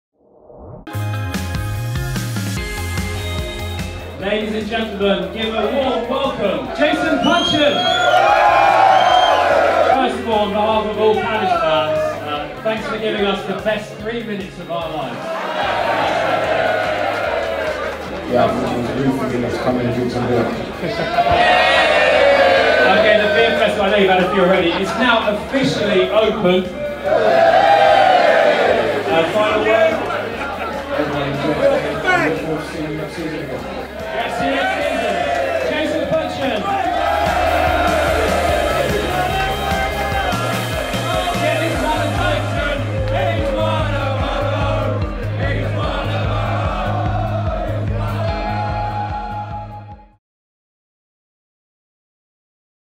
CPFC Beer Festival 2016
Jason Puncheon opens the CPFC Beer Festival.